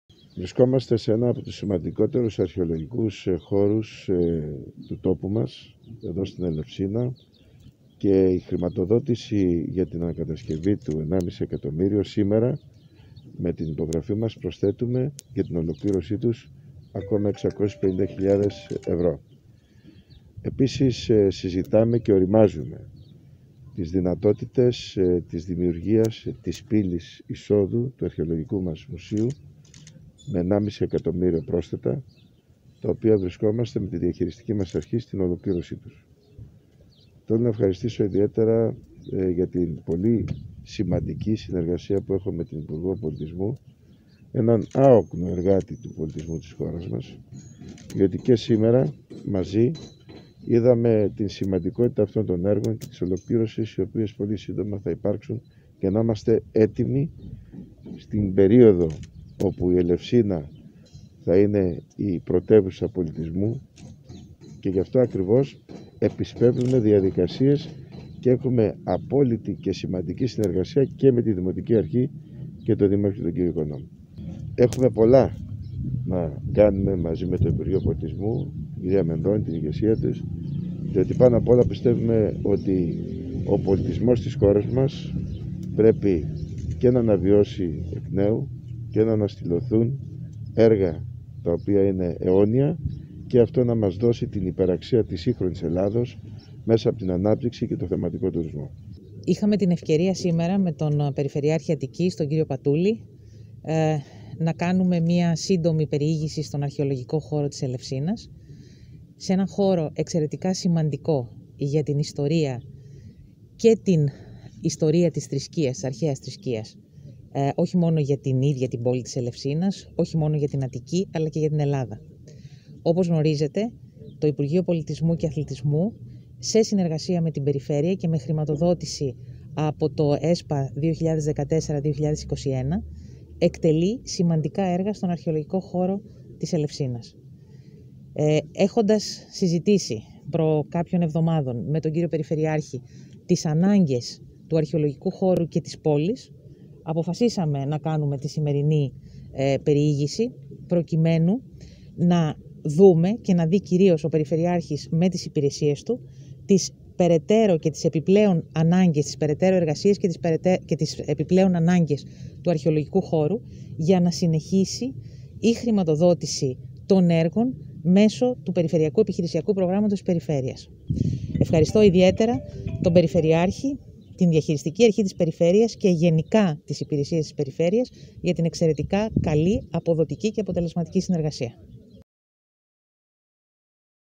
Επίσκεψη στον Αρχαιολογικό χώρο Ελευσίνας | Γ.Πατούλης 15-4-2021
Επίσκεψη με την Υπουργό Πολιτισμού και Αθλητισμού Λίνα Μενδώνη στον Αρχαιολογικό χώρο Ελευσίνας όπου πραγματοποιούνται έργα που χρηματοδοτούνται από το ΠΕΠ Αττικής